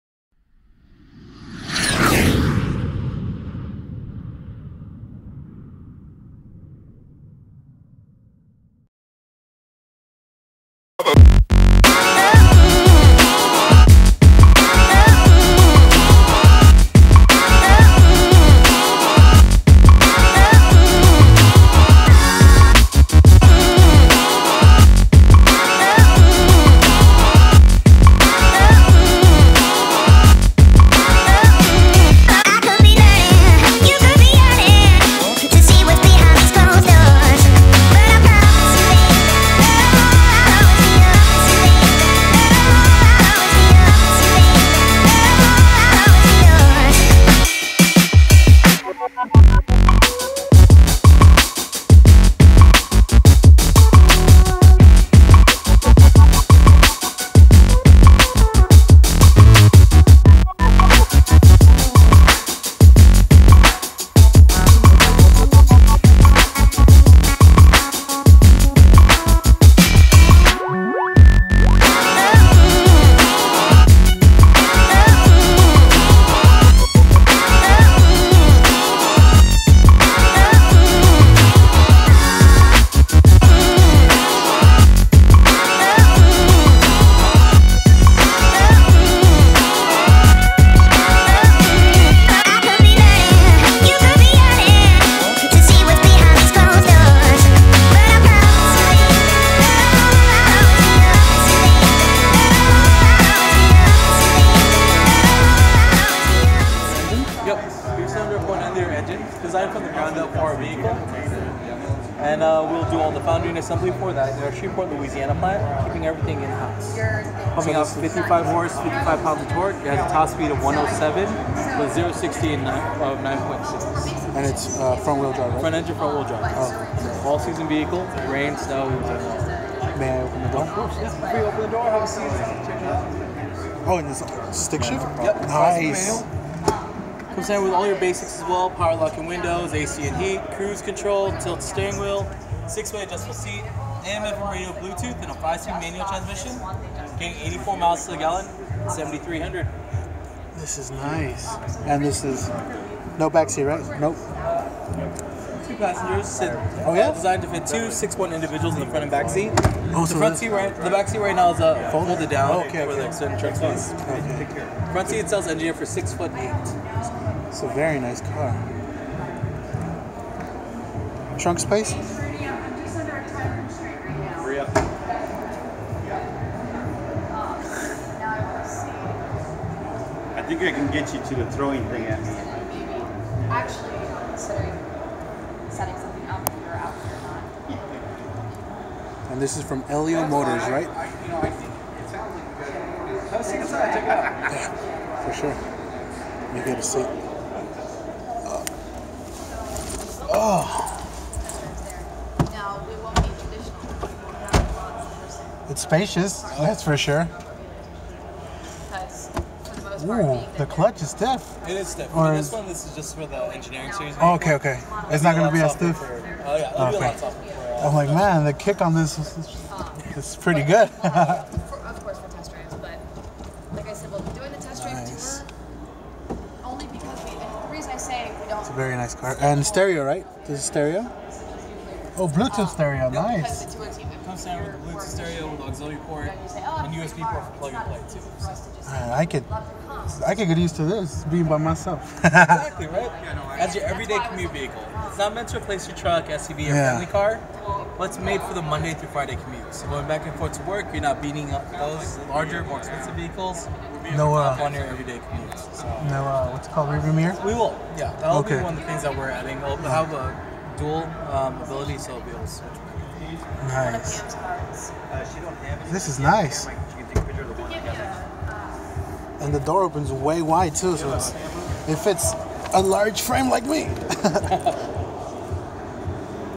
Elio Motors 2016 LA AutoShow
Elio Motors is having a great idea when it comes to creating a small car with a very exclusive amount of customizable features as well as cheap the starting Price is $7,800